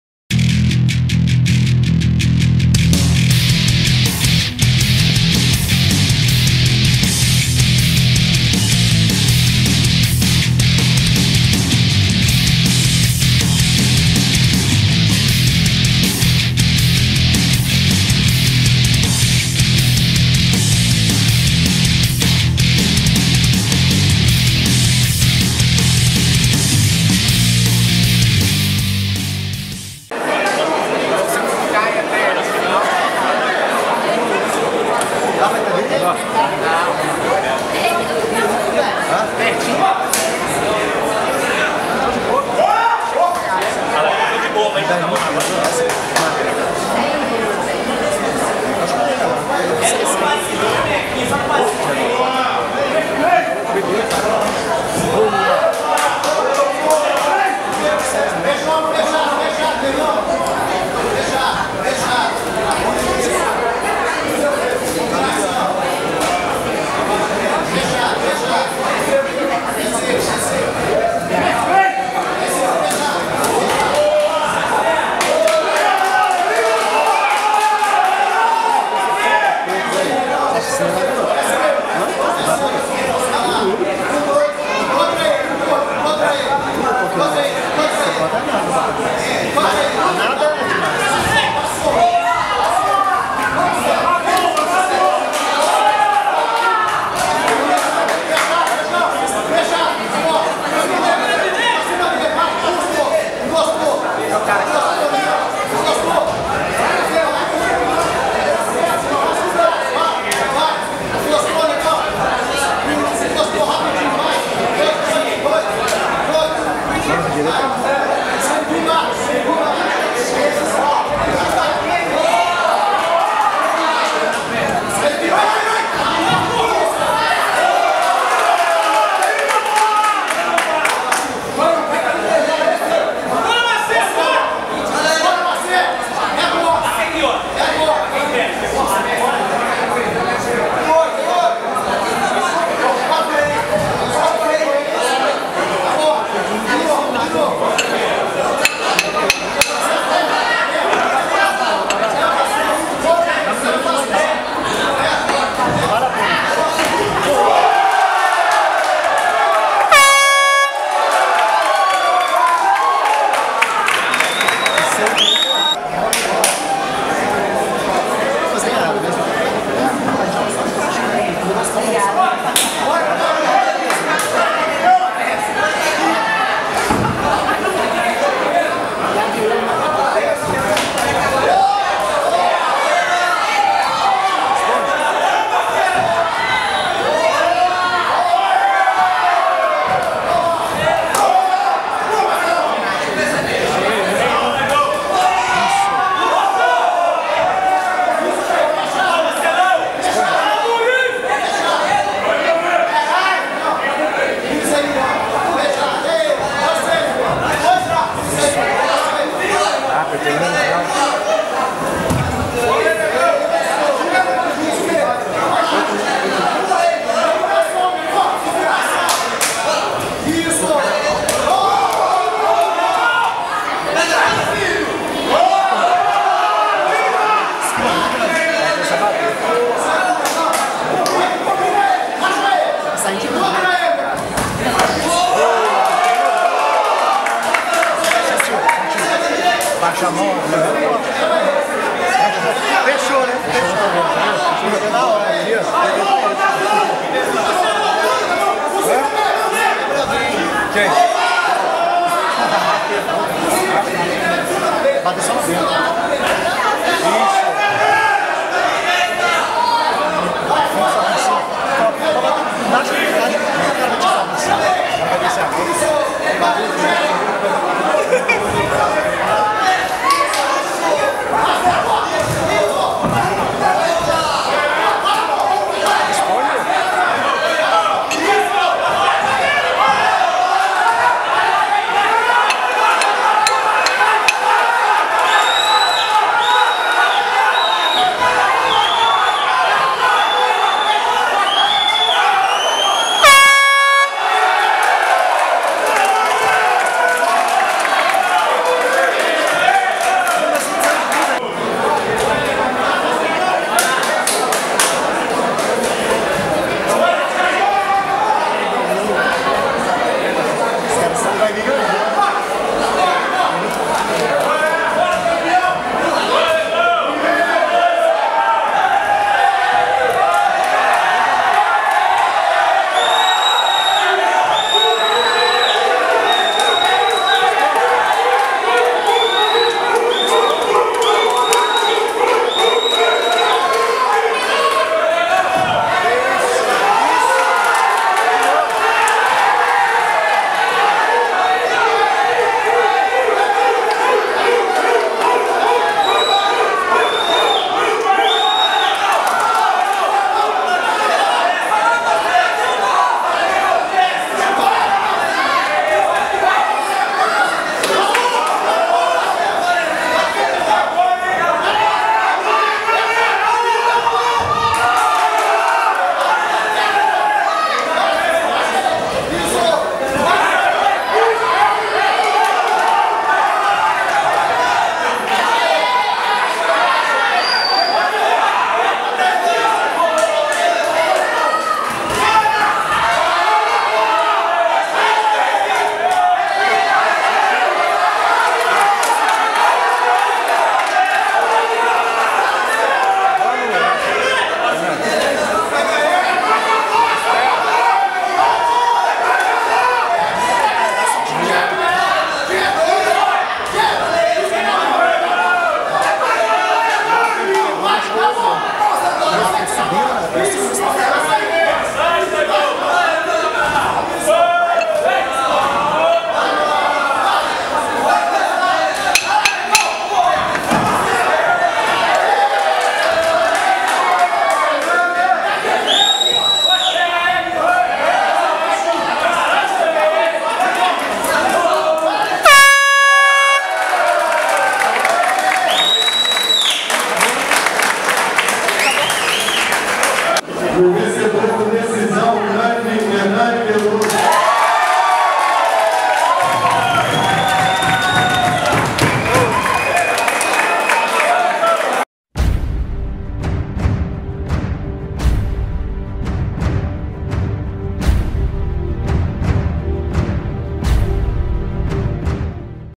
Hernane vs Marcelo - BH Sparta 9
Hernane Veloso (HV Boxe) venceu Marcelo Santos (Gui Boxe Team) por pontos.